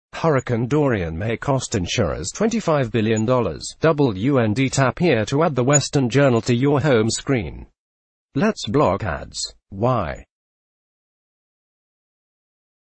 Hurricane Dorian may cost insurers $25 billion - WND
Hurricane Dorian may cost insurers $25 billion - WND Tap here to add The Western Journal to your home screen. Let's block ads! (Why?)